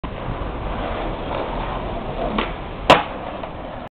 4 escaleras, men!
yo saltando 4 escaleras de la plaza de la reina de en palma.. 11/o1/o8